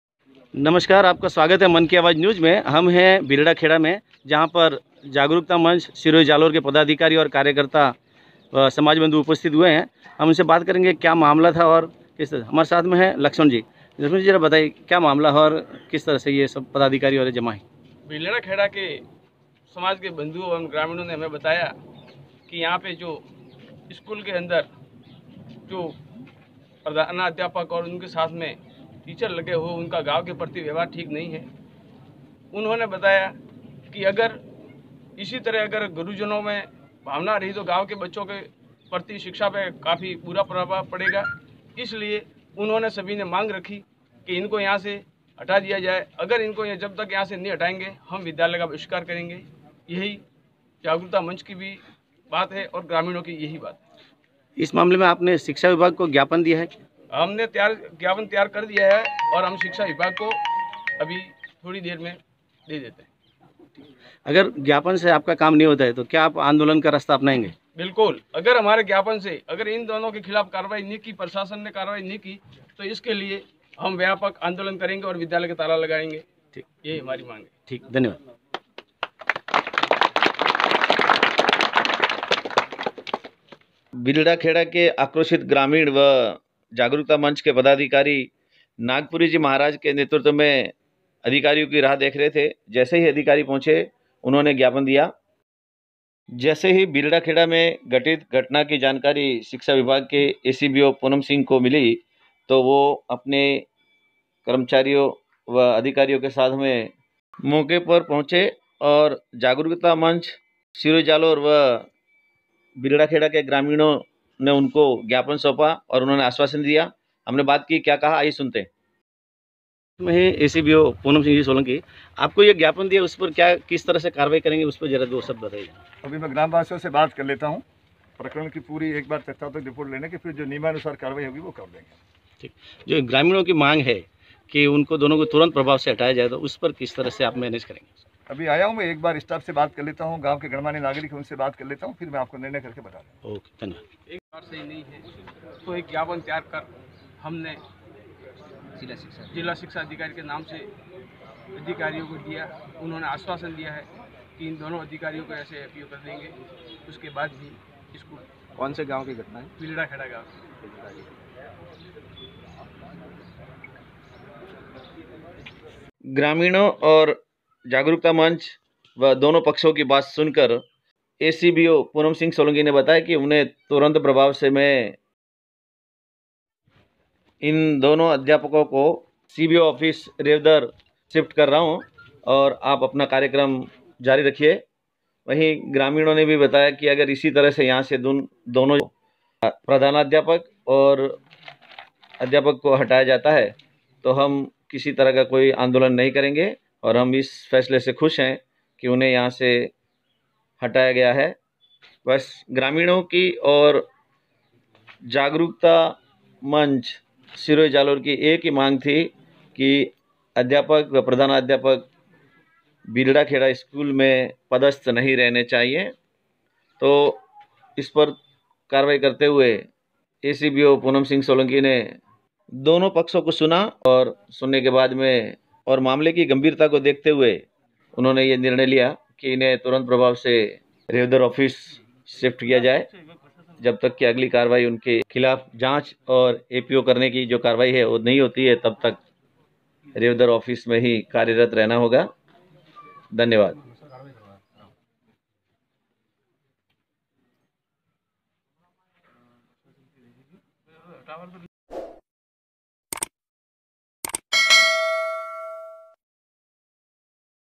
भीलडा खेडा अध्यापक मामले पर कार्यवाही करते हुए अध्यापक व प्रधानाध्यापक को CBO off. रेवदर सिफ्ट किया
भीलडा खेडा अध्यापक मामले पर कार्यवाही करते हुए Acbo पूनम सिंह सोलंकी ने तुरंत प्रभाव से कार्यवाही करते हुए आरोपी अध्यापक और साथ ही साथ प्रधानाध्यापक को CBO off. रेवदर कार्यालय सिफ्ट किया। जो अगली कार्यवाही तक रेवदर ऑफिस मे ही कार्यरत रहेंगे।